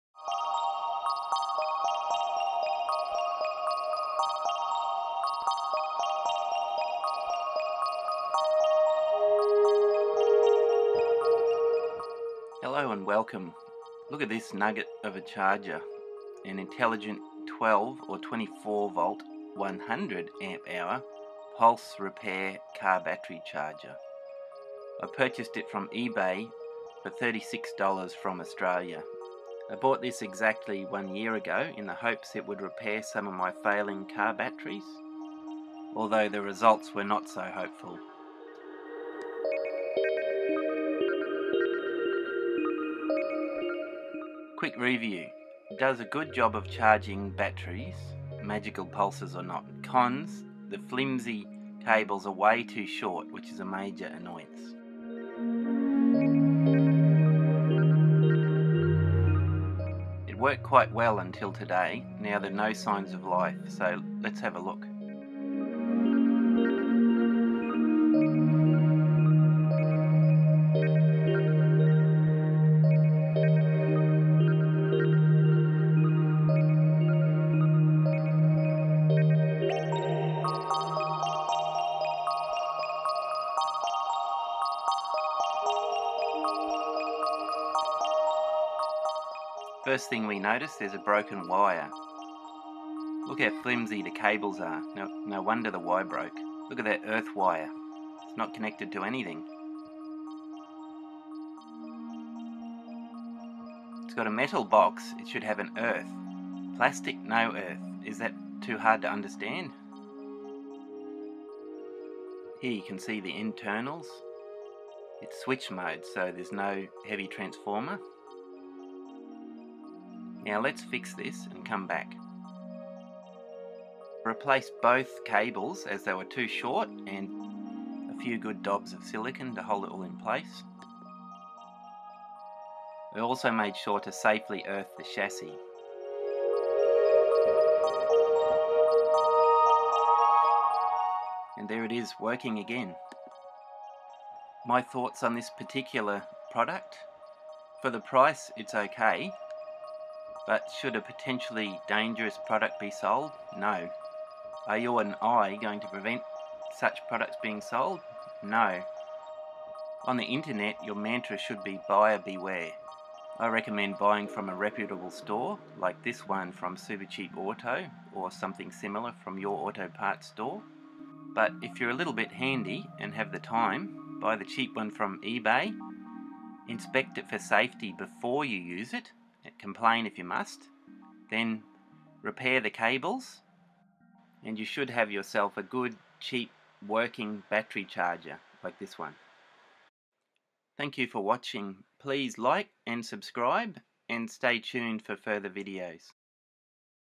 XW 20S Battery Charger Had A Really Nasty Fault!
Believe it or not my opinion has completely changed. This repaired charger is still going strong 2 years later. I use its flexible output voltage to charge my power wall when it's been raining a lot. It is a really good charger!

You may have noticed I blurred the first word which was actually "Chinesium", I believe this is a racist and derogatory term and that kind of thing is not who I am. China doesn't have the monopoly on shoddy unsafe products or workmanship, any tradesman or manufacturer can endanger peoples lives by cutting corners or just plain making mistakes, even yourself. Again the onus is on the buyer, if you want to be duped by junk, no matter which country it came from the fault is on you for buying it.
For the record this charger was not junk, it just had a major problem which I easily resolved. After my repair this charger still works (as of 4+, years later)  it's my main battery charger and I've even used it for charging 28V lithium ion batteries!

Links:
XW 20S eBay.com
XW 20S eBay Australia
Similar model on AliExpress